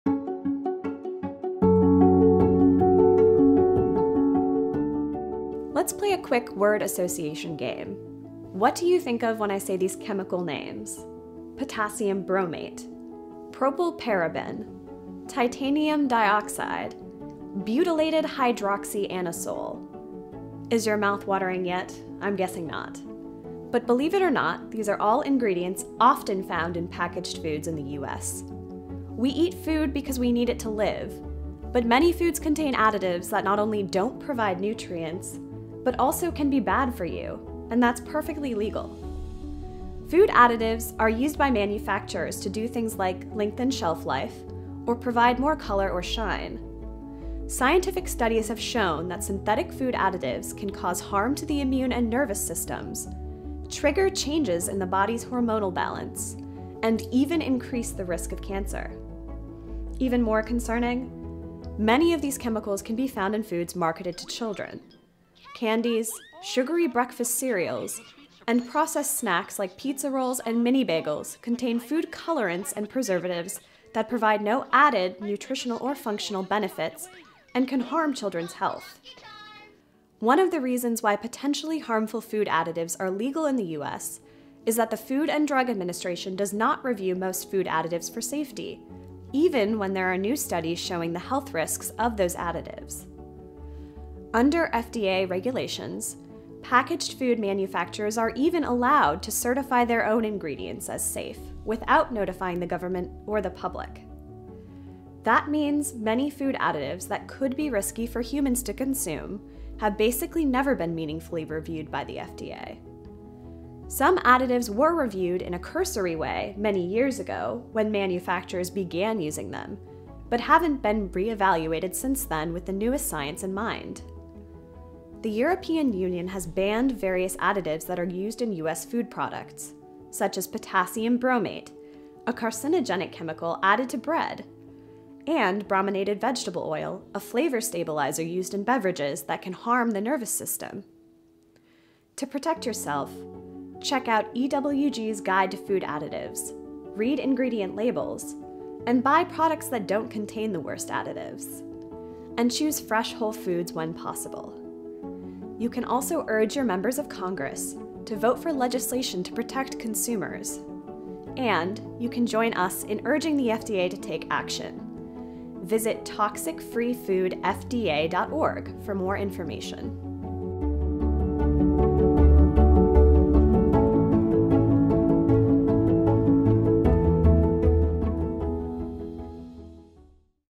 Food additives: More than you may have bargained for
We eat food because we need it to live. But many foods contain additives that not only don’t provide nutrients, but also can be bad for you – and that’s perfectly legal. Food additives are used by manufacturers to do things like lengthen shelf life or provide more color or shine.

Scientific studies have shown that synthetic food additives can cause harm to the immune and nervous systems, trigger changes in the body’s hormonal balance, and even increase the risk of cancer.

Even more concerning? Many of these chemicals can be found in foods marketed to children. Candies, sugary breakfast cereals and processed snacks like pizza rolls and mini bagels contain food colorants and preservatives that provide no added nutritional or functional benefits and can harm children’s health.

One of the reasons why potentially harmful food additives are legal in the U.S. is that the Food and Drug Administration does not review most food additives for safety, even when there are new studies showing the health risks of those additives. Under the FDA regulations, packaged food manufacturers are even allowed to certify their own ingredients as “safe” without notifying the government or the public.

That means many food additives that could be risky for humans to consume have basically never been meaningfully reviewed by the FDA. Some additives were reviewed in a cursory way many years ago when manufacturers began using them, but haven’t been re-evaluated since then with the newest science in mind.

To protect yourself, check out EWG’s Guide to Food Additives, read ingredient labels, and buy products that don’t contain the worst additives. And choose fresh, whole foods when possible.

Join us in urging the FDA to take action. Visit toxicfreefoodfda.org for more information.